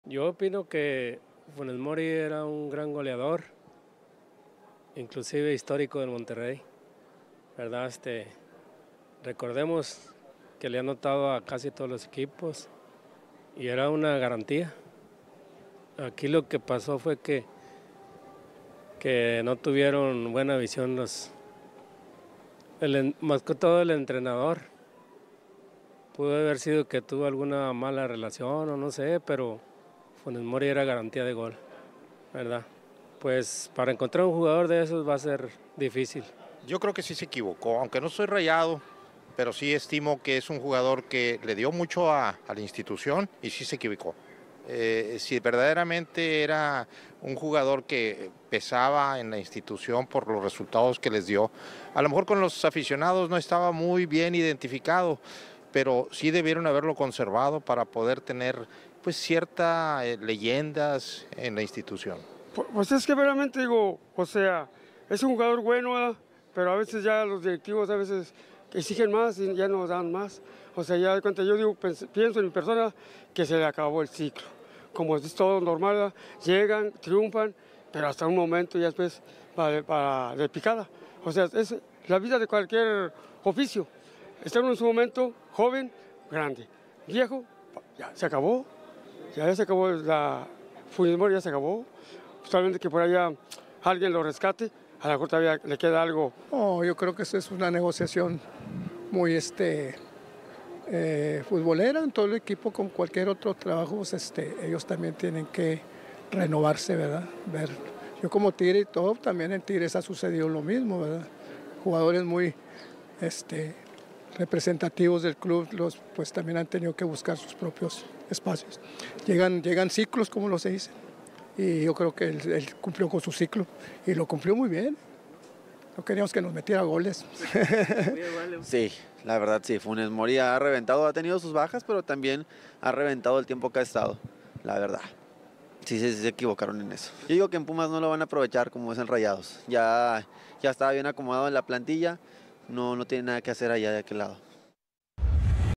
¿Directiva Rayada se equivocó al no renovar a su goleador histórico? | Pasión Futbolera
Sondeo sobre futbol regio.